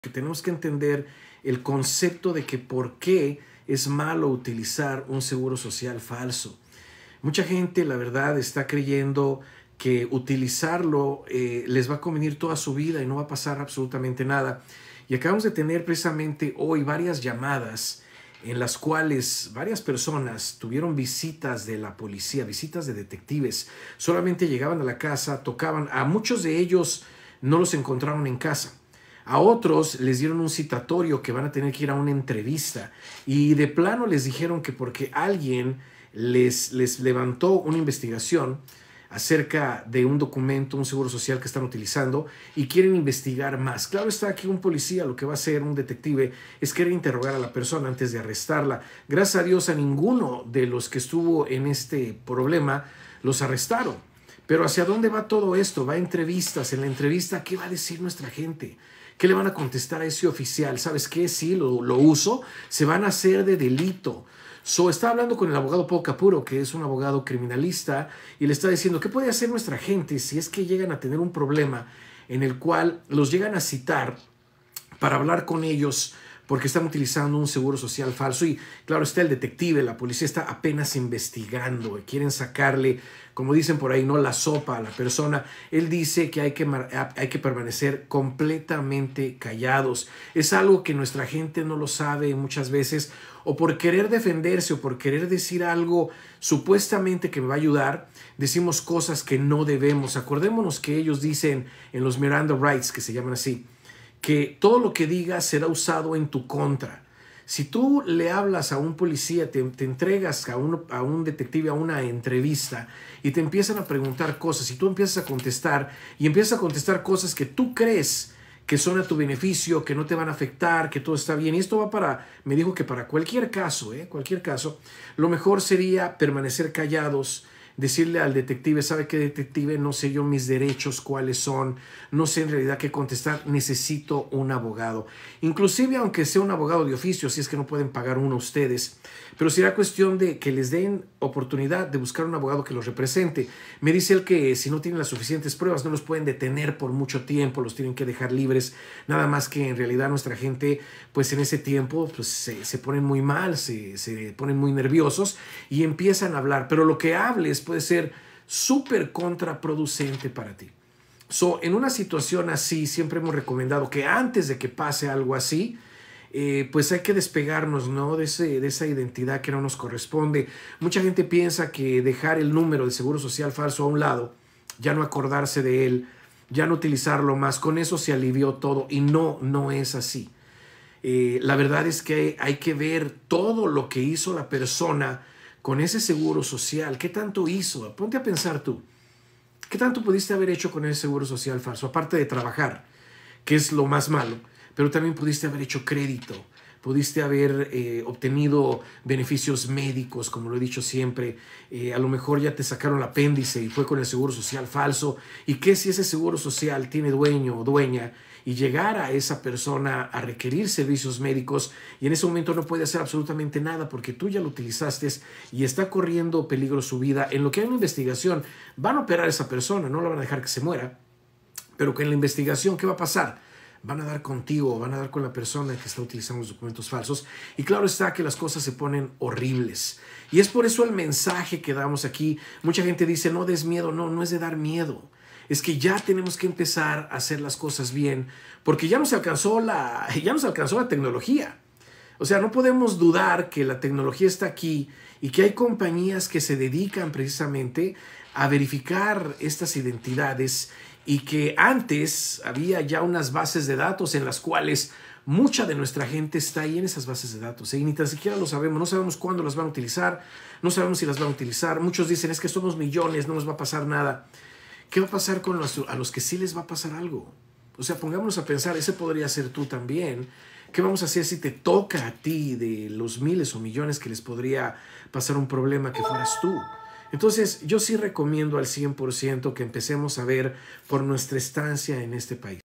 INMIGRANTE ¿Porque es malo utilizar un Seguro Social Falso?
INMIGRANTE  ¿Porque es malo
utilizar  un Seguro Social Falso?

Si tienes un caso legal siempre consulta con un abogado. Tony Flores no es abogado

Llama Ahora